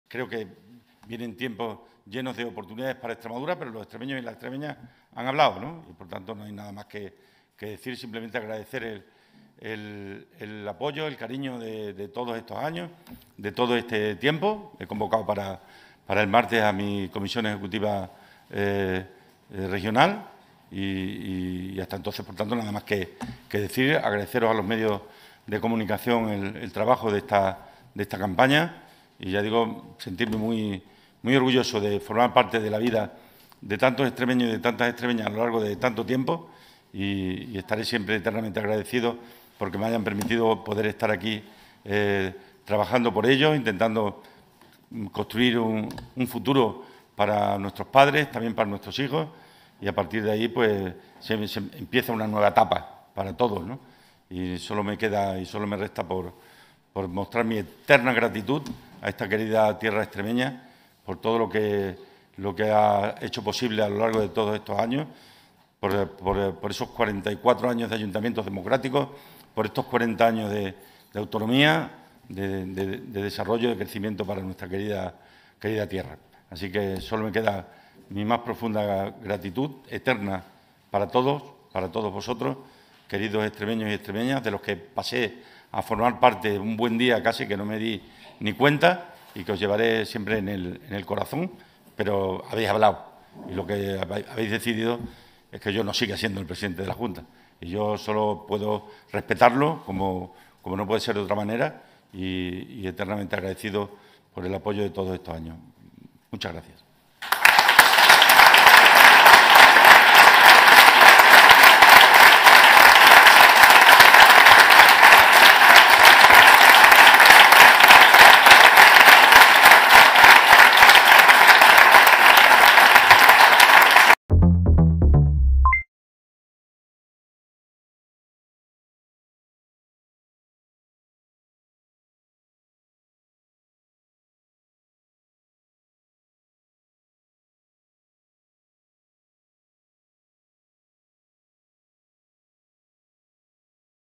Vara (PSOE) pierde Extremadura y convoca a la Ejecutiva regional: "Os llevaré en el corazón"
El bloque de la derecha, liderado por la candidata del Partido Popular María Guardiola, ha ganado las elecciones autonómicas en Extremadura en este 28M, gracias a los 28 diputados del PP y los cinco de Vox. Así el candidato del PSOE y actual presidente de la Junta de Extremadura Guillermo Fernández Vara no sólo pierde la mayoría absoluta sino también la presidencia.”Yo he fracasado, es una realidad obvia”, ha dicho Vara al inicio de su comparecencia de prensa.

El bloque de la izquierda suma 32 diputados, 28 del PSOE, que pierde seis con respecto a 2019, y 4 de Unidas por Extremadura, que mantiene la misma representación que hace cuatro años.De ser investida presidenta de la Junta de Extremadura, Guardiola será la primera mujer que se sitúe al frente del Ejecutivo de esta comunidad autónoma.